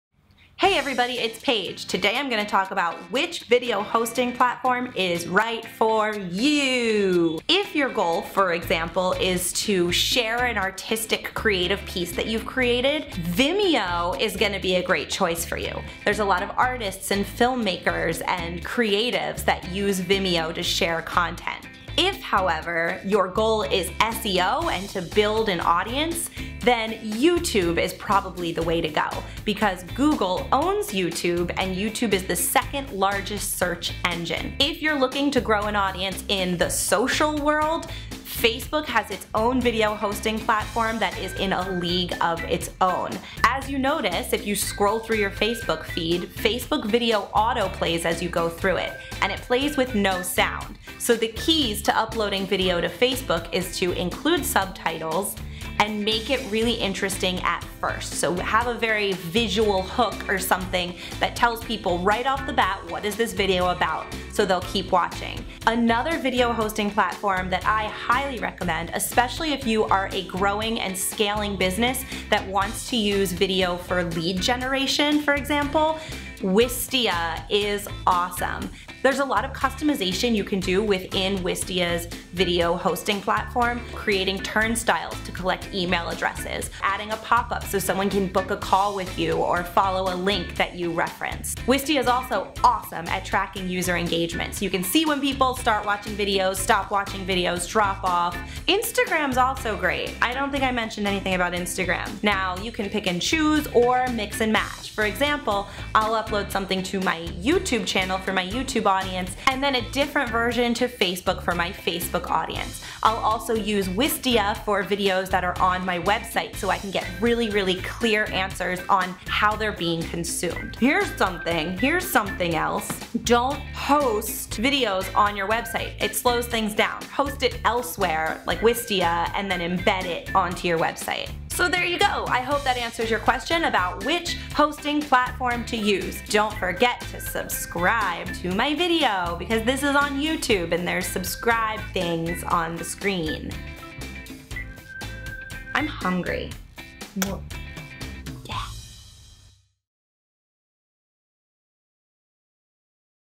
Which Video Hosting Platform Should You Use?
So many platforms, so little time! How do you know which video hosting platform is right for you? That depends on your goal with the video content. This video is hosted on YouTube, but there are lots of other options out there. Watch the video and tell me what you think! Which platform is your favorite?

And don't forget about The Influencer Studio where you can learn about branding, creating video content, and editing tips  ⇣